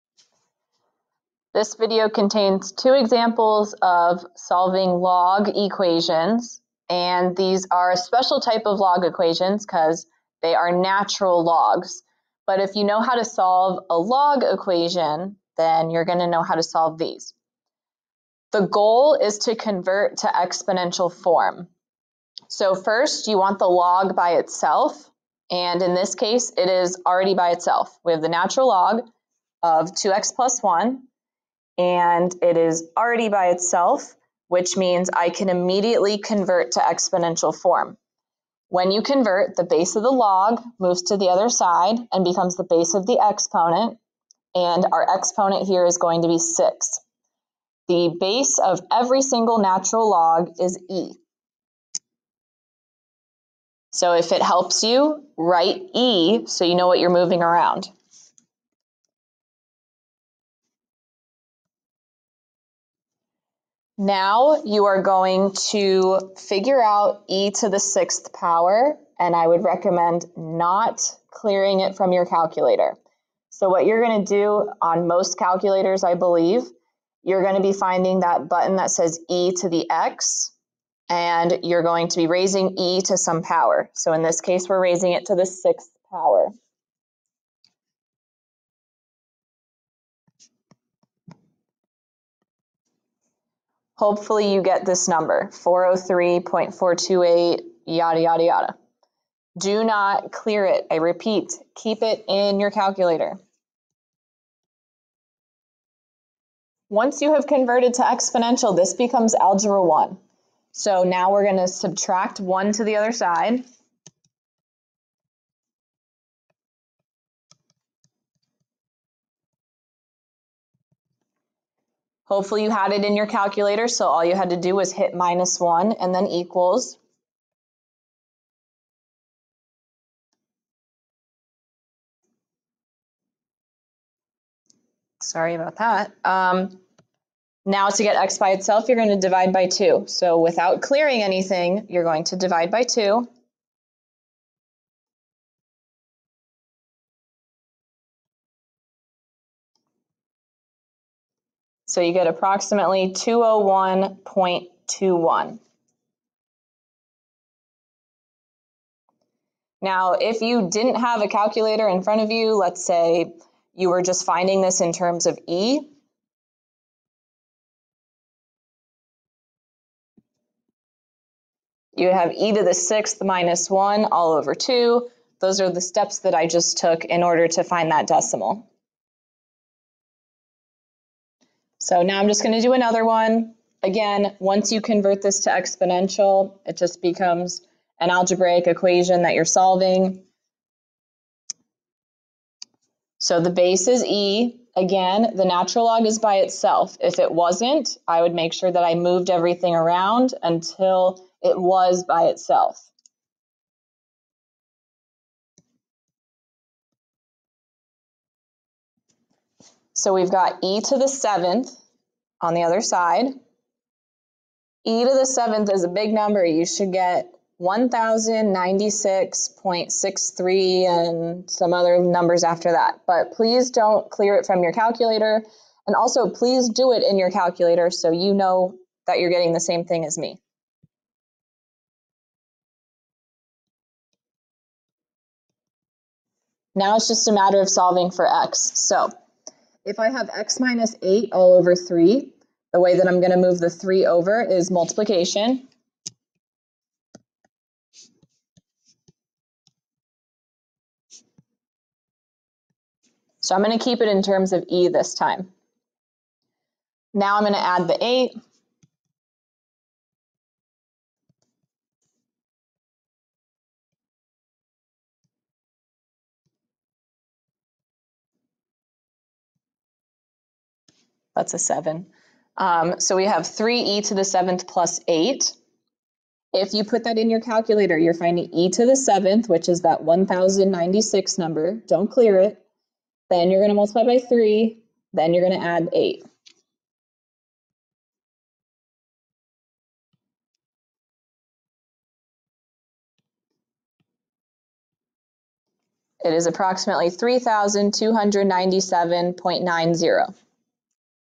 Solving Natural Log Equations
How to solve for x when an equation contains a natural log